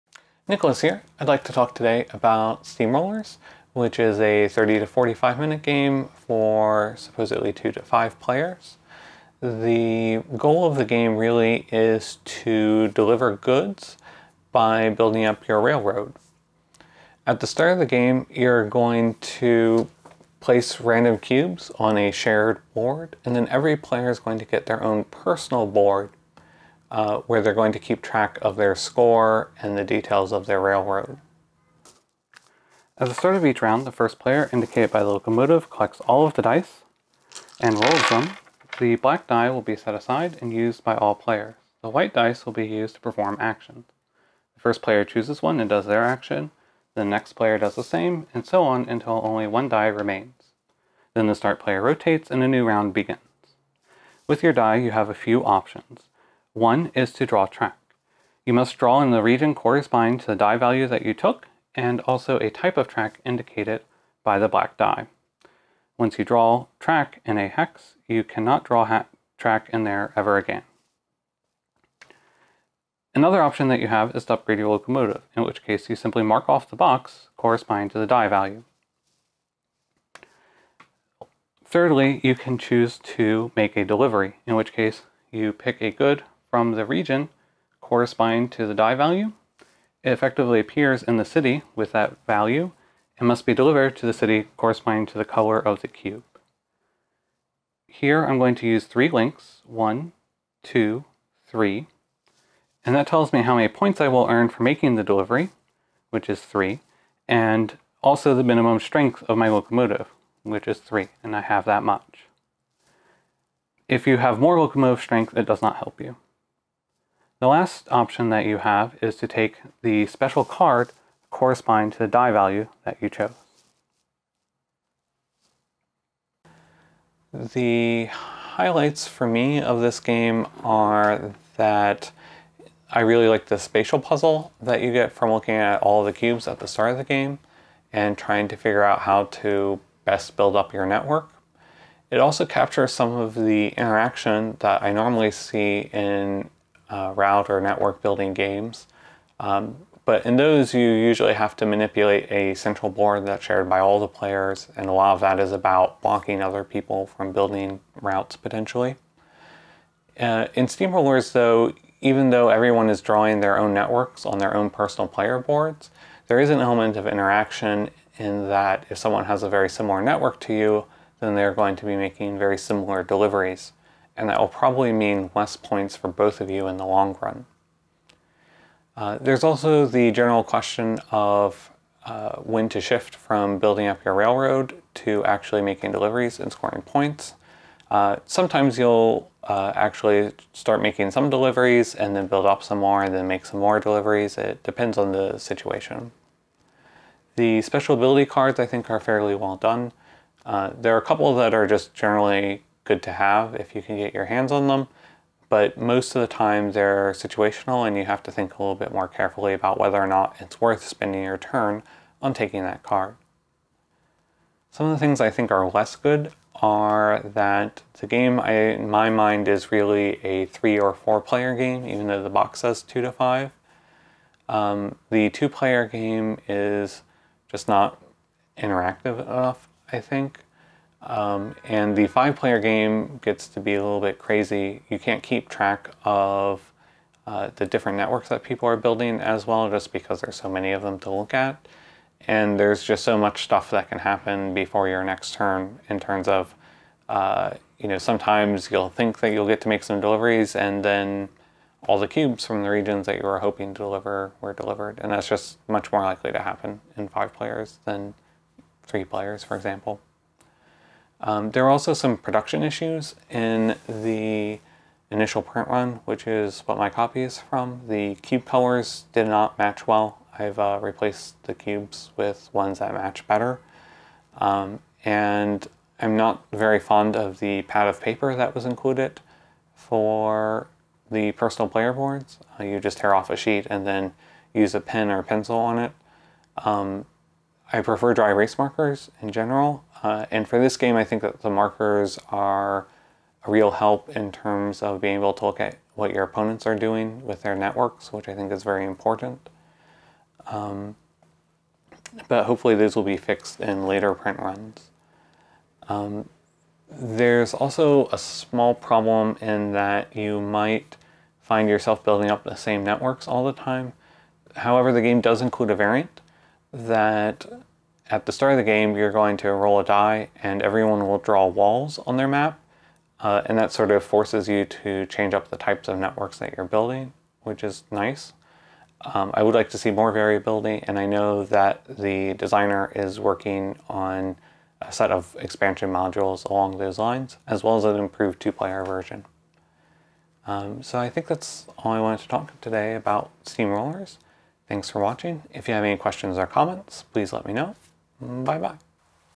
SteamRollers Review with Nikolas
Nikolas reviews of SteamRollers, a dice game where you build your rail network and use it to deliver goods.